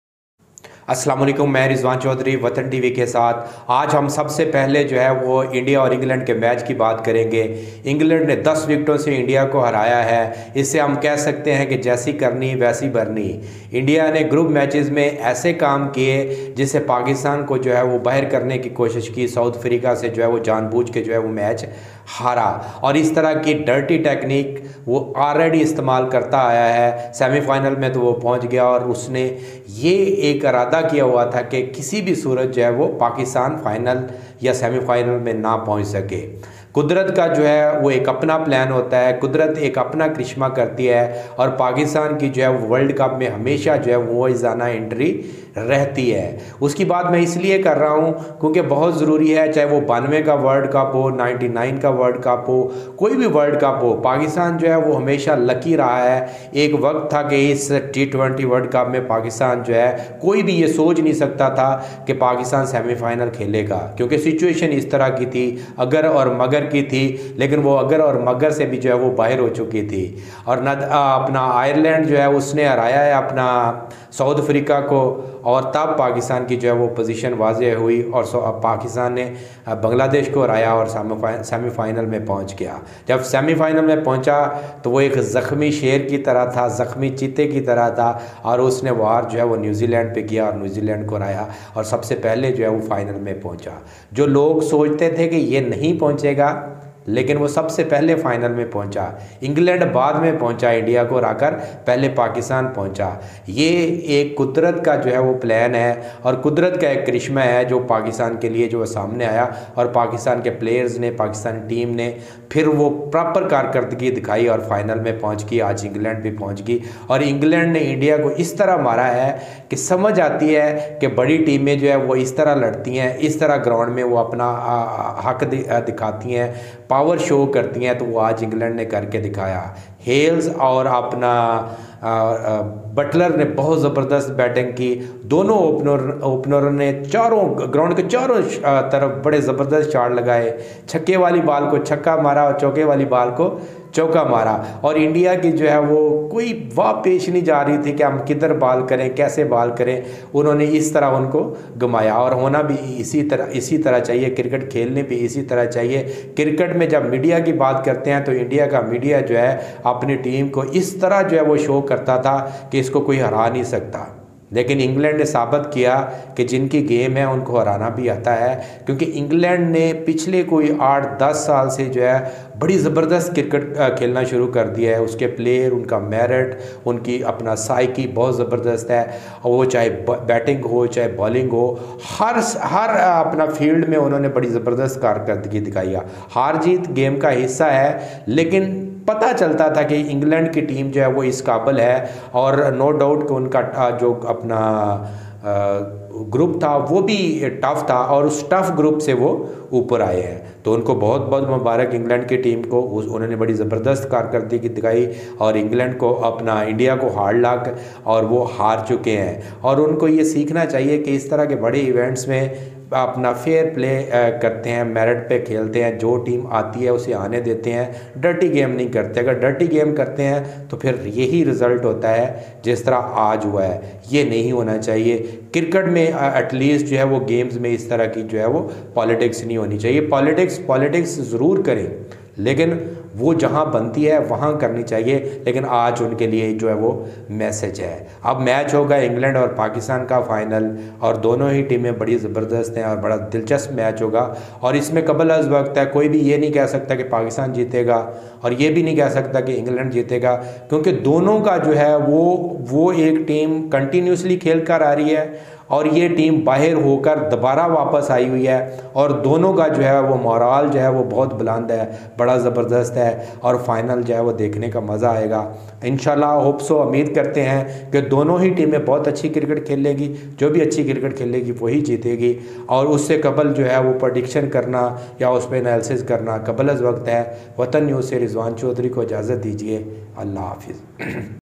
Pakistan vs England T 20 world cup Final Match 2022 will be on Sunday| England won by ten wickets
Pakistan vs England T 20 world cup Final Match 2022 will be on Sunday| England won by ten wickets| Watan TV international| Rizwan Chaudhary| live match|